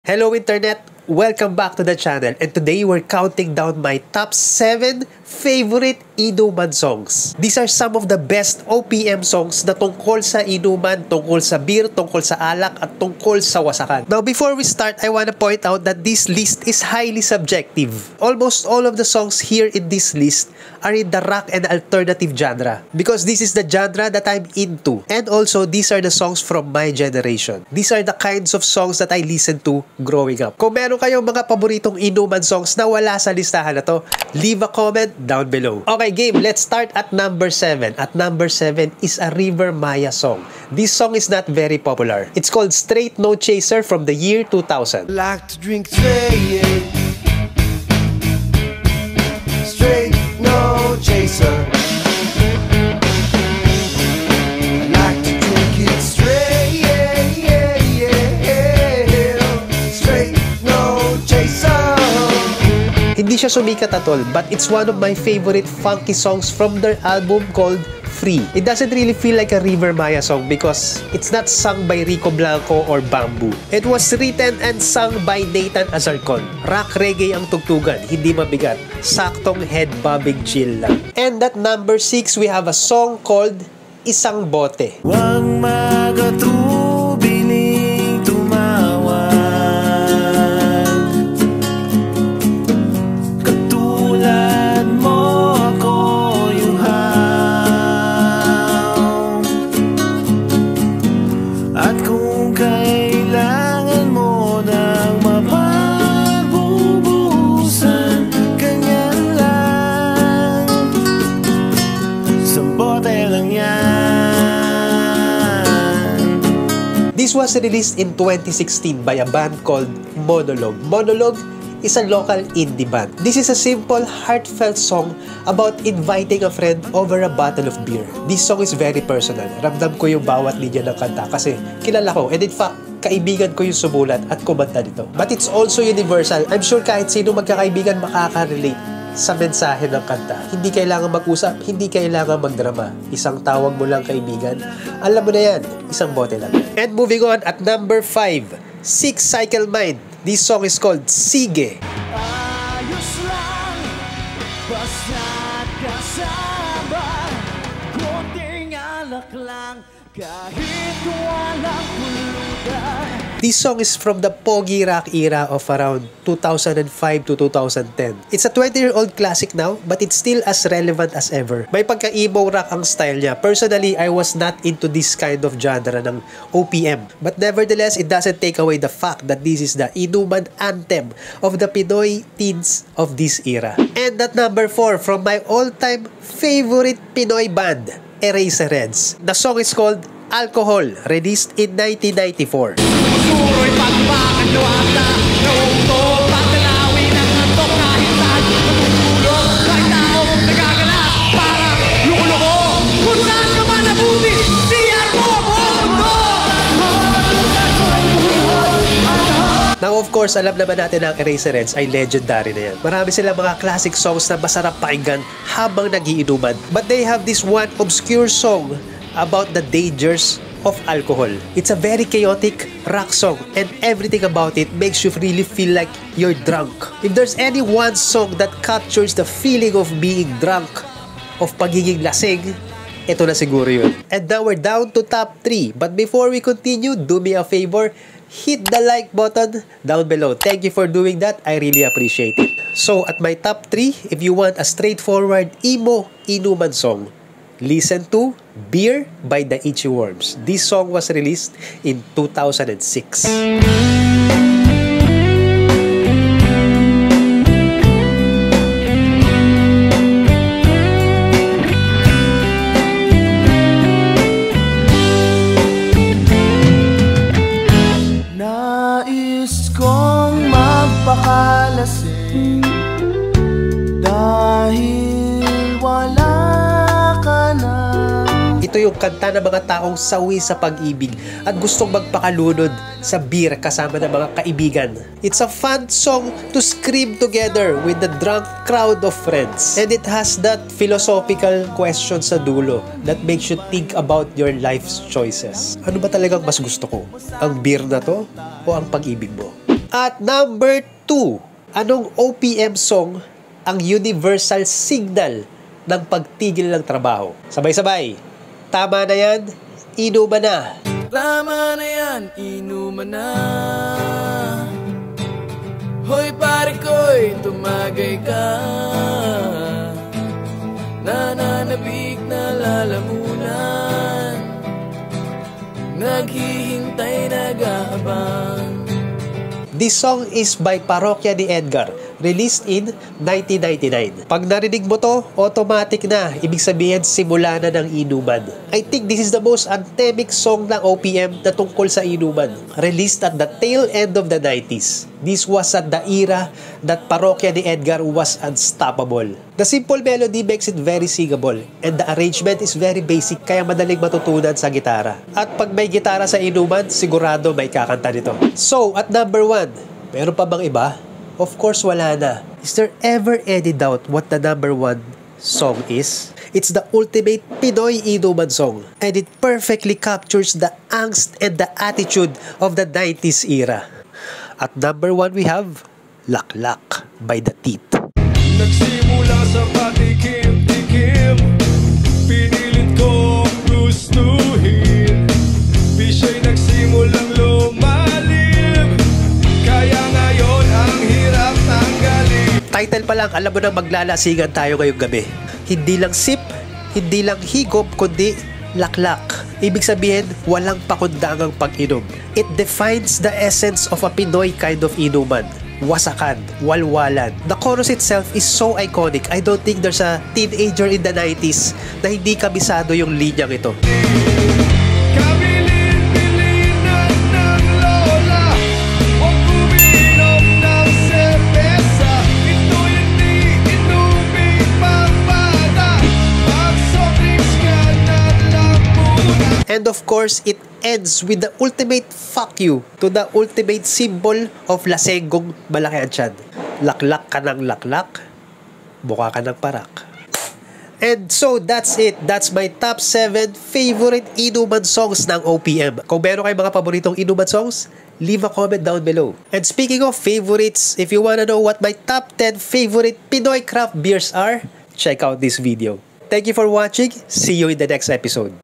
Best songs to get DRUNK to if you’re a 90s Teenager
Teenager ka ba Nung 90s? Tara jamming! Basta inuman, asahan mo na may kantahan. These are MY top seven favorite OPM songs that talk about the topic of alcoholism. 90s Pinoy Bands that defined a generation. Kung may paborito kayo na wala dito, lapag nyo sa comments!

eraserheads
rivermaya
6cyclemind
parokya ni edgar
monolog
the teeth
itchyworms